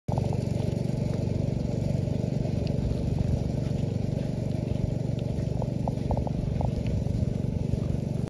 Quality over Quantity every single day
Hand rolling Zeon Zoysia Sods ensuring that only the best quality gets to our clients.